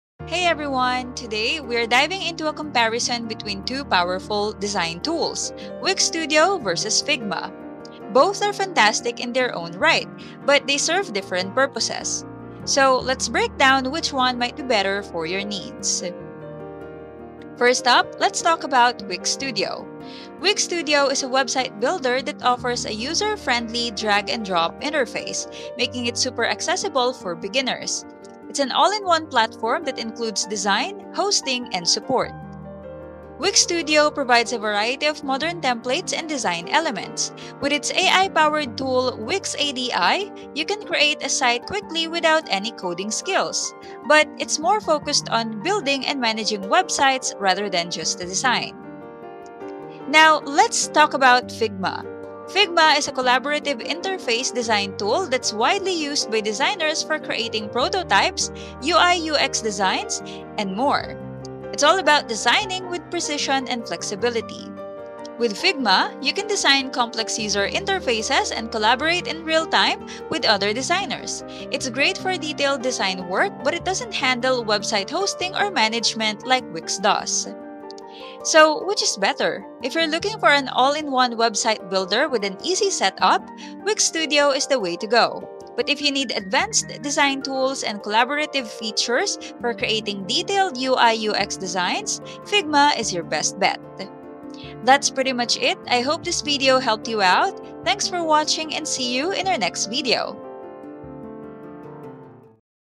Wix Studio vs Figma || Which is Better?
In this video, we'll compare Wix Studio and Figma to help you decide which design tool is better for your projects.